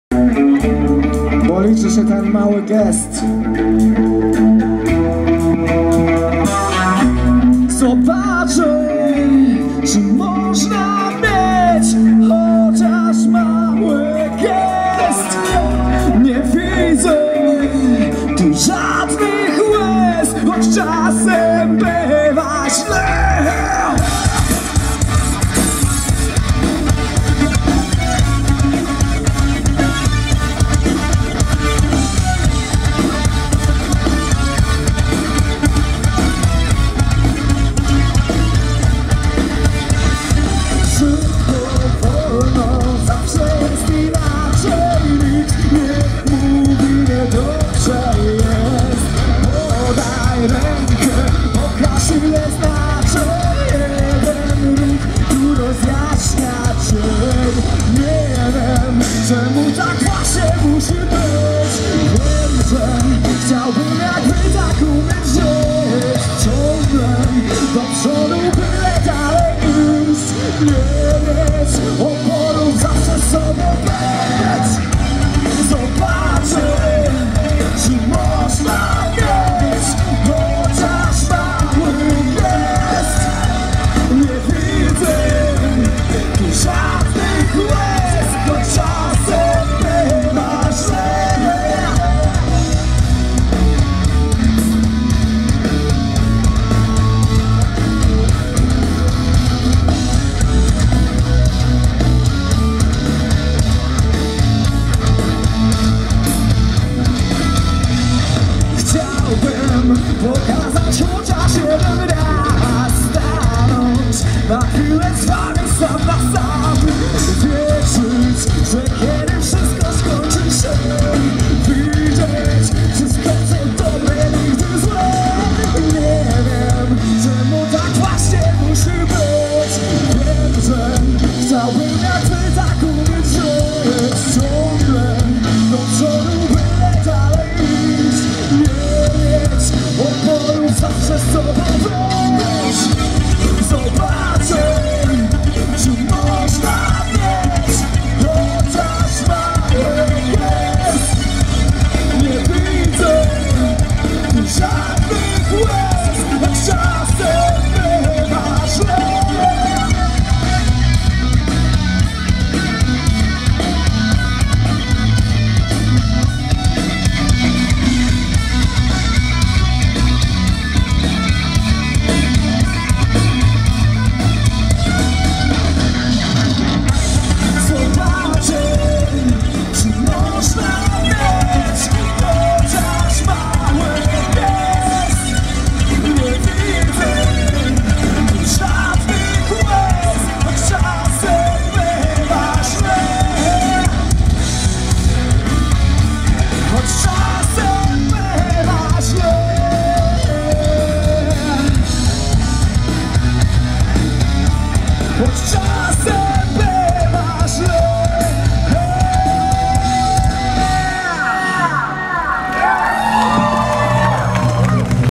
Carrion - Klub M (Radom, 14.01.18, WOŚP)
Koncert zespołu Carrion w Radomiu podczas 26. Finału Wielkiej Orkiestry Świątecznej Pomocy. 14 stycznia 2018 roku.